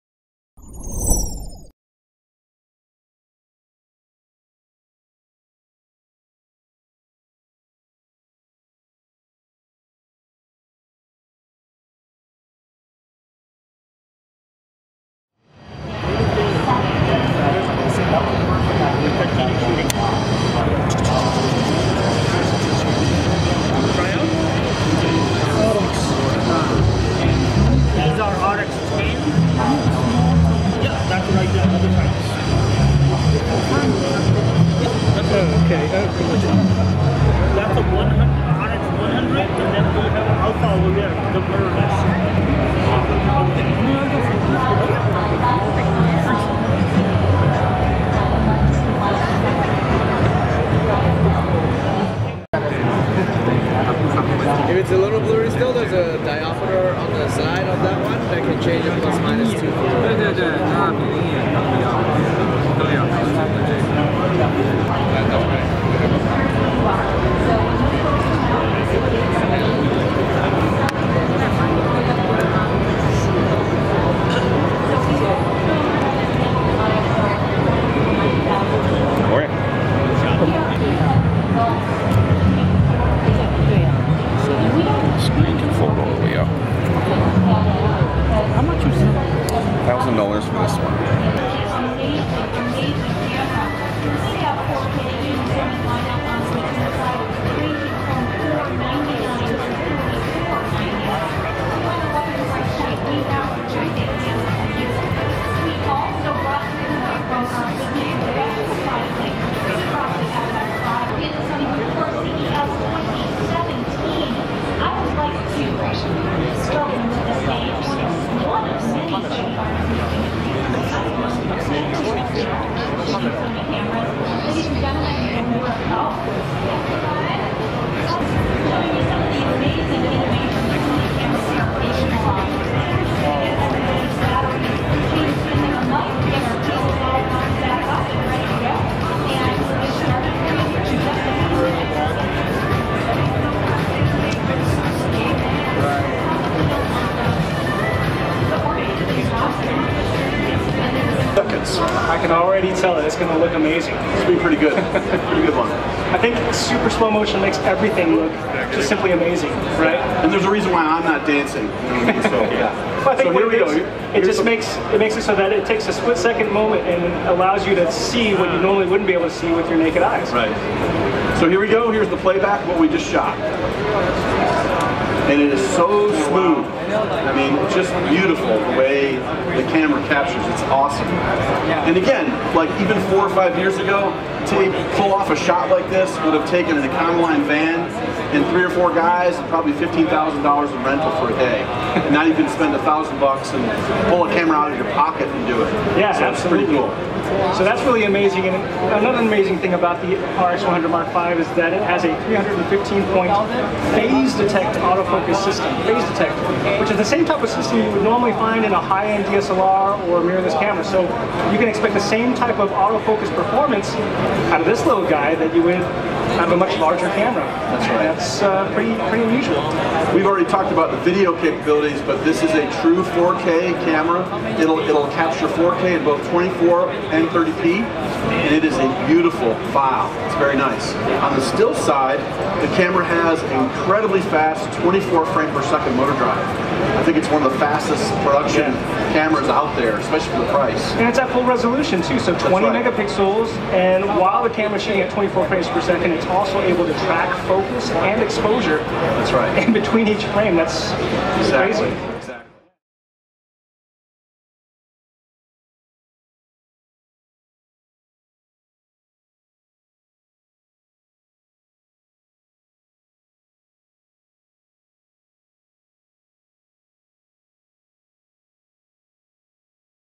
CES 2017 | Sony RX100 Mark V 4K Compact Camera | XGA OLED Viewfinder | SmartReview.com
SmartReview.com  attended the Consumer Electronics Show (CES 2017) to preview Sony's latest Cameras.  The Sony RX100 Mark V  is a new camera capable of still photos and 4K Ultra HD video.  Its feature packed, but small and compact. Features a 24-70mm  f/1.8-2.8. lens,  Manual Control Ring, 20.1 megapixel 1-inch sensor, 24 fps burst mode,  AF 315 phase detection points,  Optical SteadyShot image stabilization,  Pop-up flash,  Built-in WiFi & NFC, touchscreen,  up to ISO 12800 ,  XGA OLED popup Viewfinder,  UHD 4K Video Recording,  XAVC S codec records at up to 100Mbps.

This pocket sized wonder comes priced at just under $1000.